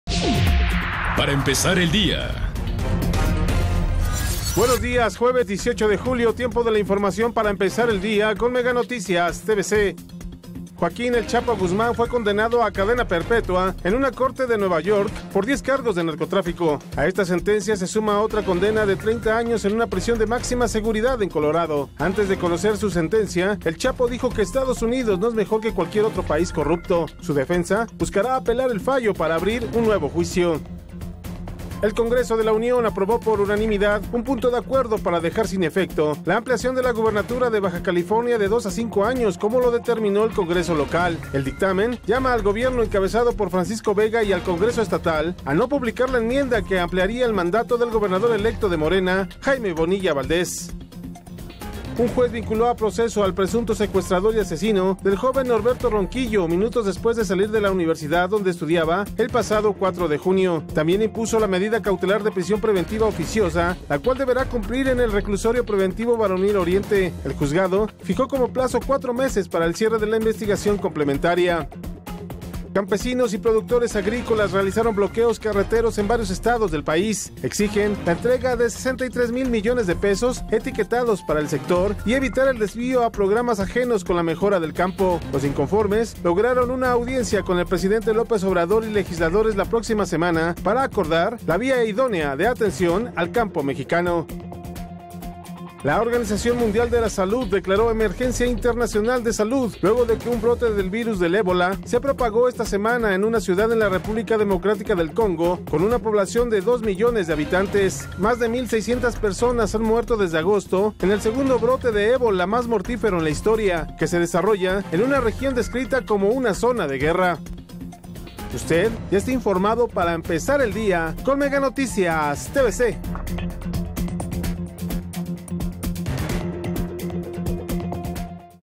El fin del 'Chapo': cadena perpetua en la prisión más dura de EU
¡Fue una inquisición! "Estados Unidos es un país corrupto, sin justicia", dijo el capo mexicano